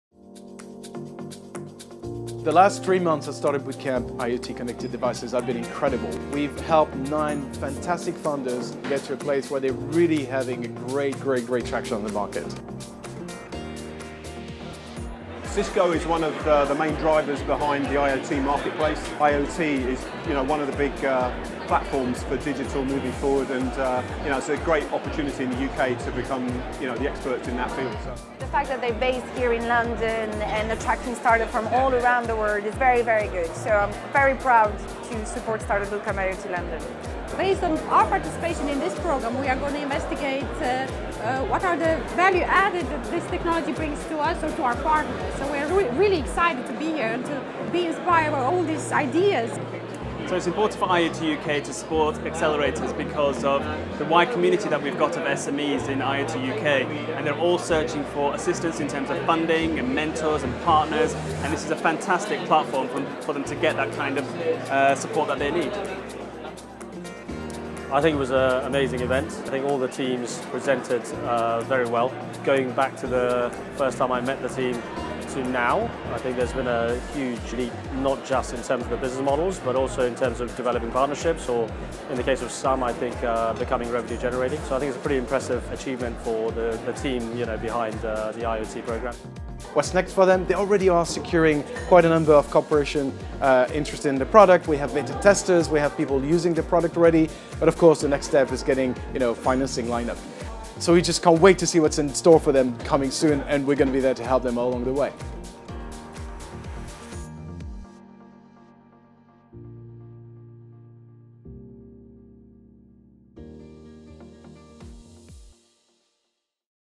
Being a Partner of Startupbootcamp IoT
On 25th January 2017 Startupbootcamp hosted their first IoT Demo Day in London with nine startups, all of whom are developing an ‘internet of things’ enabled device, pitching their companies to a room of over 300 investors, partners and mentors.
Program partners, from Cisco and DLL, to Hardware Club and IoTUK, were there to support our teams. Here's what they had to say about the program.